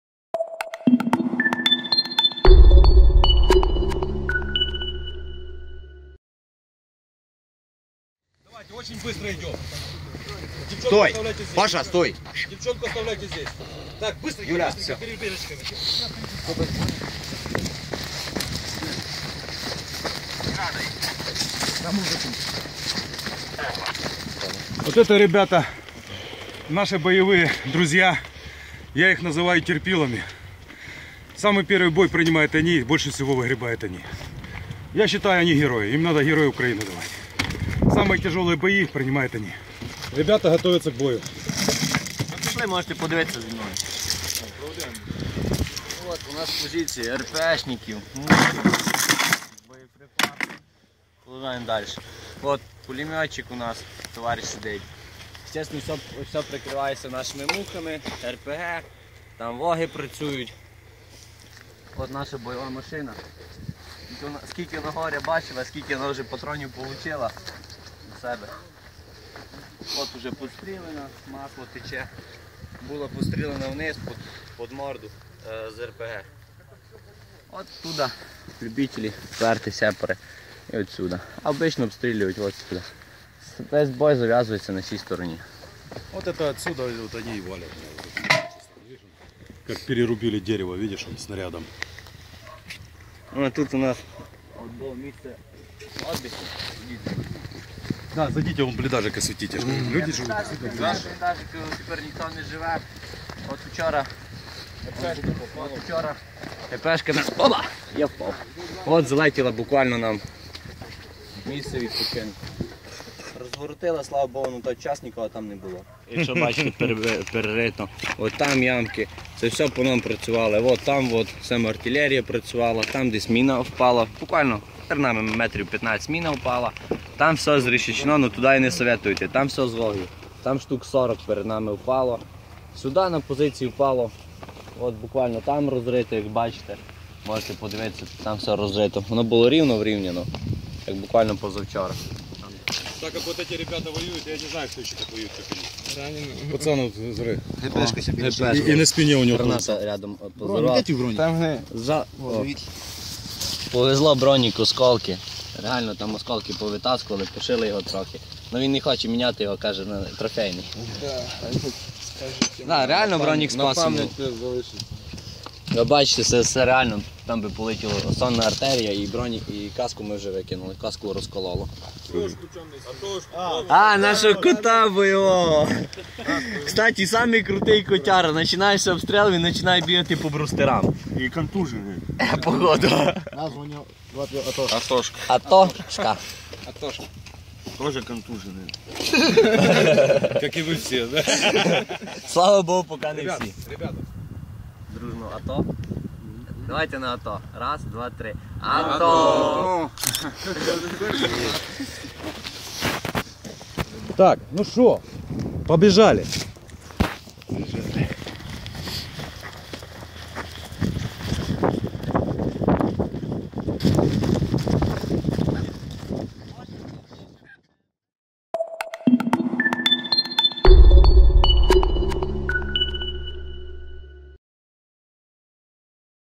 On the Front Lines with the Ukrainian Army
Ukrainian soldiers take Hromadske's film crew on a tour of their dugouts at the front lines in the Donbas region, where the bulk of recent fighting has been concentrated. In the past two weeks, these soldiers with the 128th Mechanized Infantry Brigade said they've seen an increase in shelling from the separatists in their area.
Sporadic fighting between Ukrainian and separatist forces has continued in eastern Ukraine, despite a truce that was signed back in September. Over 4,000 people including civilians, have died in the conflict since fighting broke out in April.